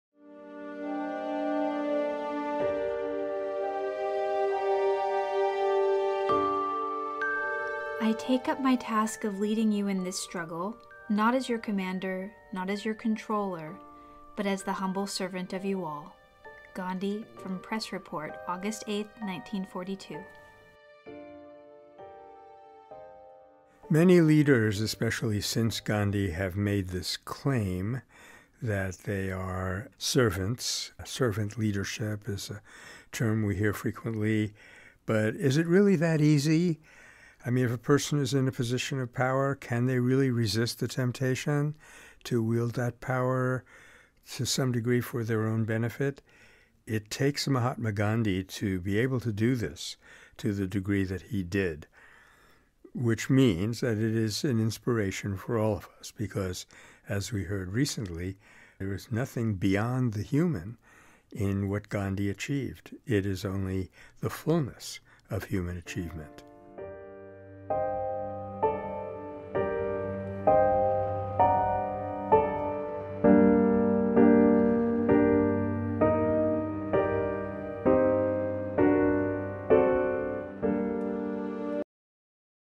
Nonviolence Daily | Day 151
I take up my task of leading you in this struggle, not as your commander, not as your controller, but as the humble servant of you all.
-Gandhi, Press Report, August 8, 1942